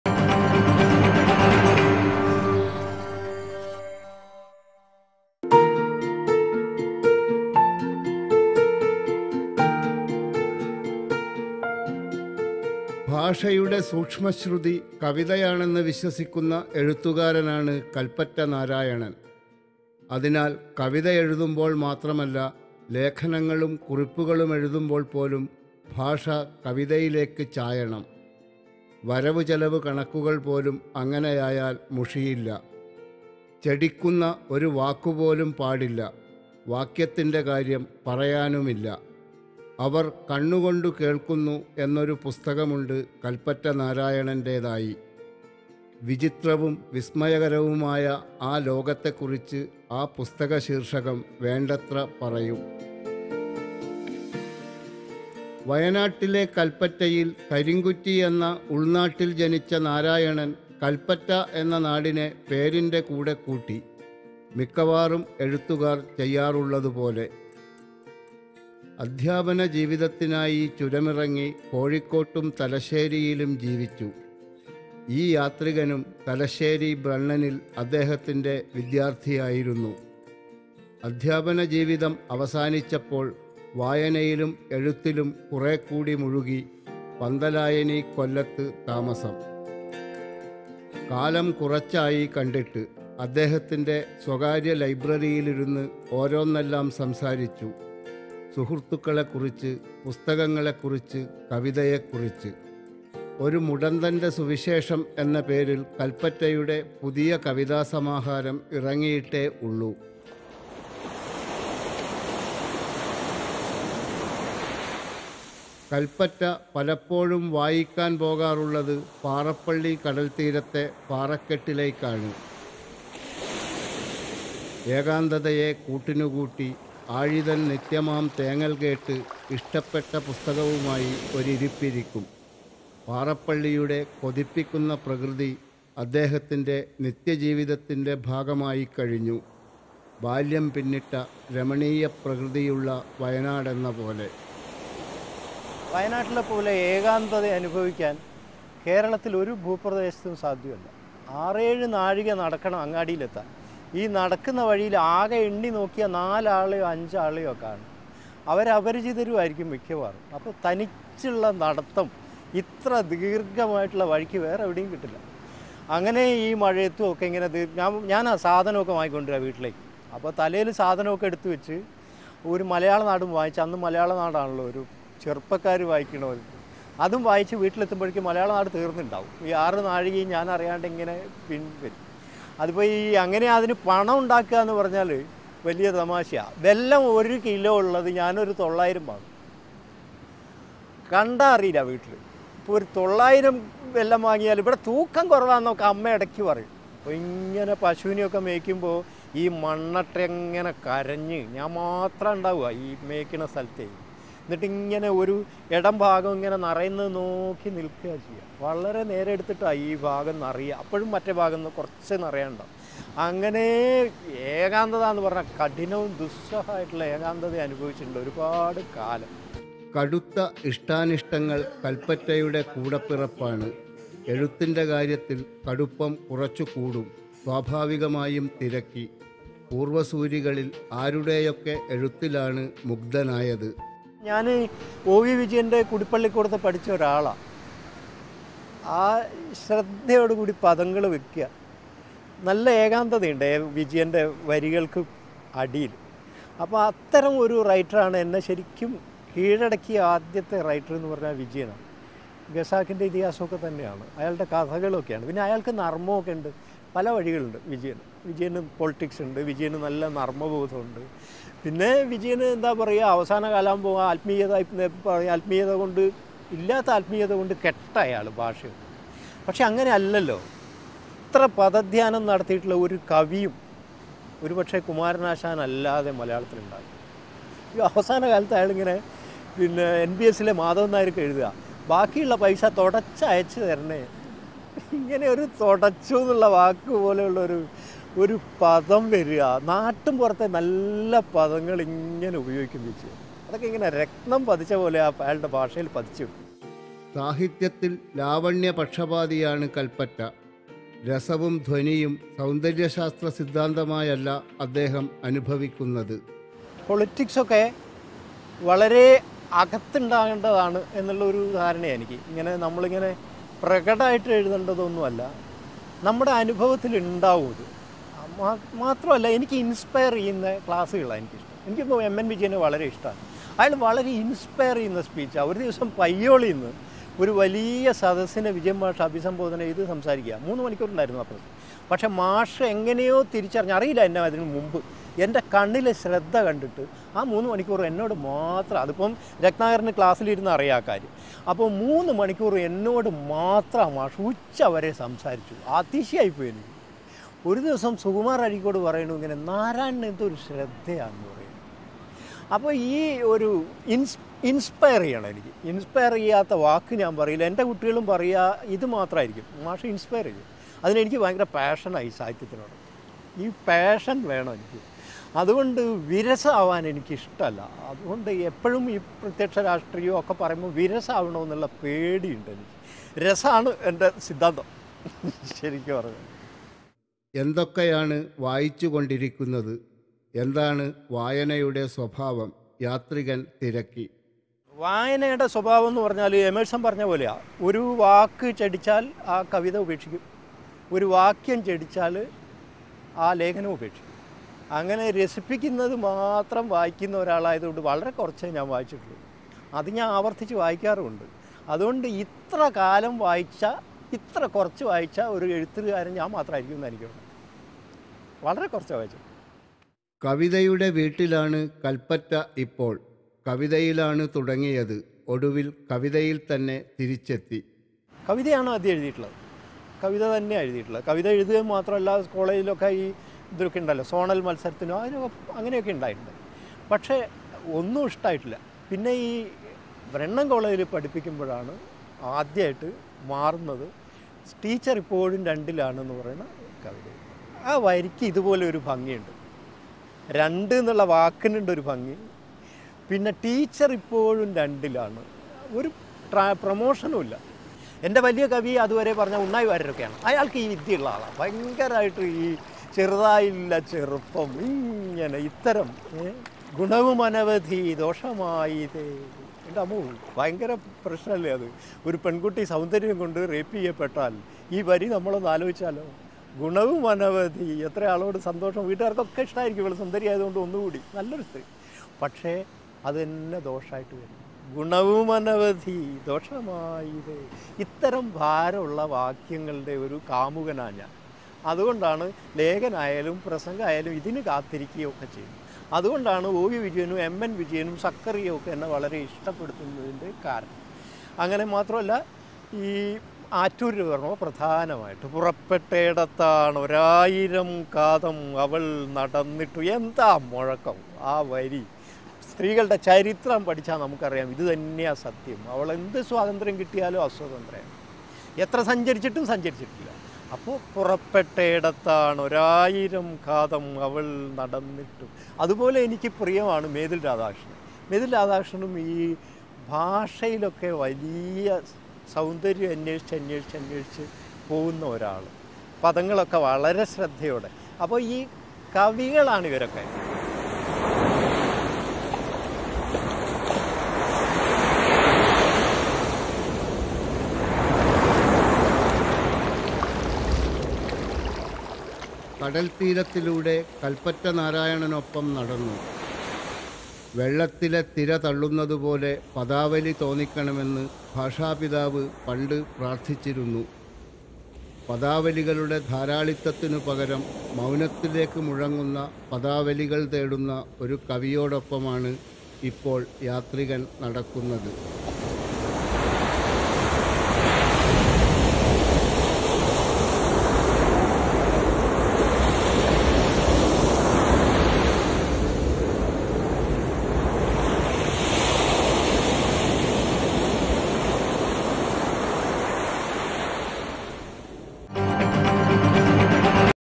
Writter Kalpetta Narayanan on Yatra 16th Aug 2013 Part 1
A travelogue by Mangad Ratnakaran. This time Mangad Ratnakaran takes us to Panthalayani Kollam, vadakara, maniyoor